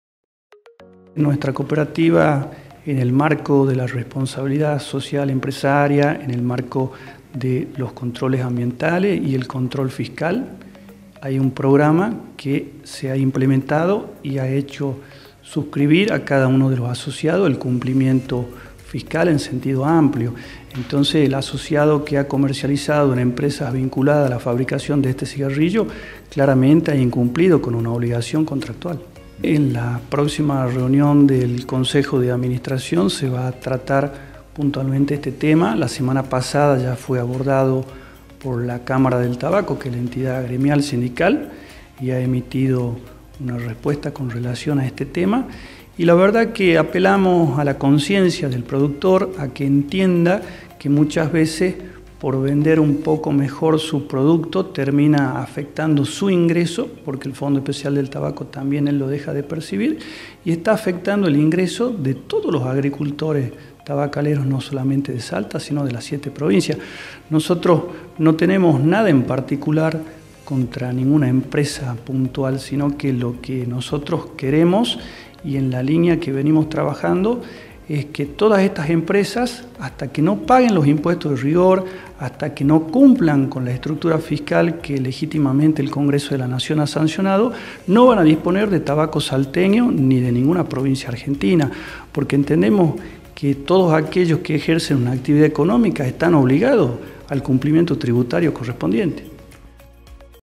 PROVEEDORES DE LA EVASIÓN. Enrique Cornejo - Dirigente Tabacalero Salteño
Queremos que todas estas empresas no van a disponer de tabaco salteño ni de ninguna provincia Argentina, hasta que no paguen los impuestos de rigor y hasta que no cumplan con la estructura fiscal que legítimamente el Congreso de la Nación  ha sancionado.